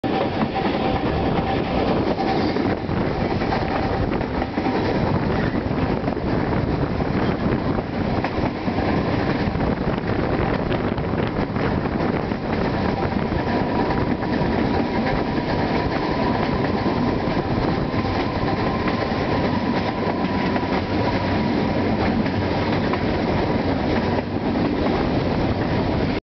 Narrow gauge (760mm) BDZ steam train (Septemvri-Velingrad)
A narrow gauge (760mm) steam locomotive of Bulgarian Railways (BDZ) was rented for a special steam trip.

The trip was organized by the Greek Friends of the Railways (SFS, on Sunday 17 March 2013

Class 609-76 steam locomotive of BDZ hauled the special train from Septemvri to Velingrad.